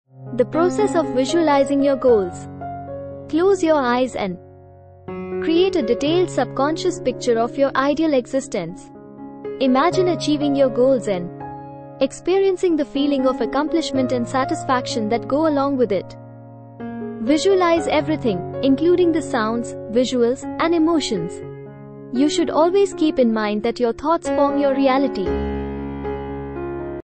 Master the Art of Visualization 🤯 | Manifest Your ❤️ Desires with Visions
Learn how to master the art of visualization and harness its power to manifest your desires with the Law of Attraction. Discover the science behind visualization, set clear intentions, and create a consistent visualization practice.
Unlock the potential of your imagination and create the life you've always dreamed of.